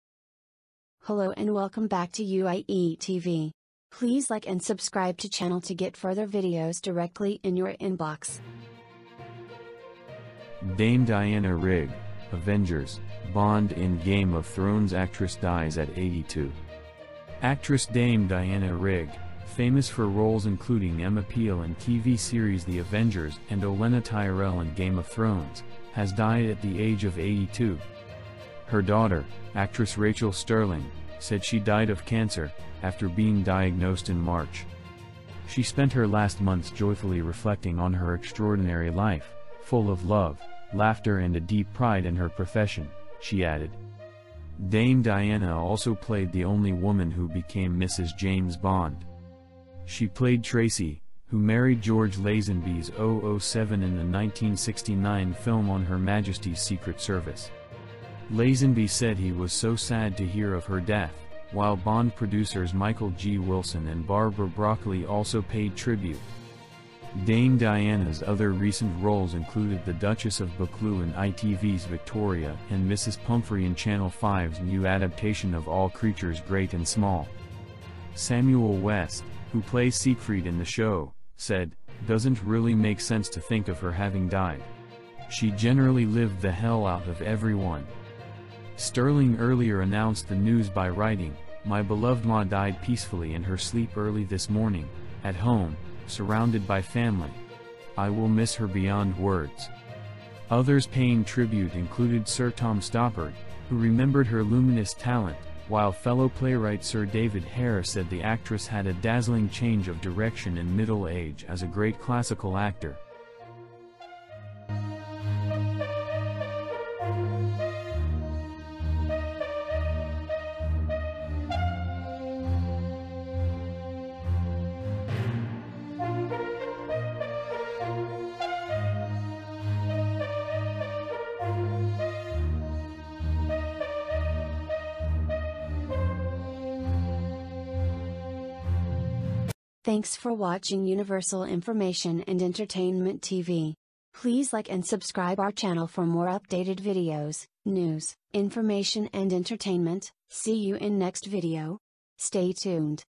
Dame Diana Rigg. Avengers, Bond and Game of Thrones actress dies at 82
Actress Dame Diana Rigg, famous for roles including Emma Peel in TV series The Avengers and Olenna Tyrell in Game of Thrones, has died at the age of 82.

Her daughter, actress Rachael Stirling, said she died of cancer, after being diagnosed in March.

"She spent her last months joyfully reflecting on her extraordinary life, full of love, laughter and a deep pride in her profession," she added.

Dame Diana also played the only woman who became Mrs James Bond.

She played Tracy, who married George Lazenby's 007 in the 1969 film On Her Majesty's Secret Service.

Lazenby said he was "so sad" to hear of her death, while Bond producers Michael G Wilson and Barbara Broccoli also paid tribute.
Dame Diana's other recent roles included the Duchess of Buccleuch in ITV's Victoria and Mrs Pumphrey in Channel 5's new adaptation of All Creatures Great and Small.

Samuel West, who plays Siegfried in the show, said: "Doesn't really make sense to think of her having died. She generally lived the hell out of everyone."

Stirling earlier announced the news by writing: "My Beloved Ma died peacefully in her sleep early this morning, at home, surrounded by family. I will miss her beyond words."

Others paying tribute included Sir Tom Stoppard, who remembered her "luminous" talent, while fellow playwright Sir David Hare said the actress had a "dazzling change of direction in middle age as a great classical actor".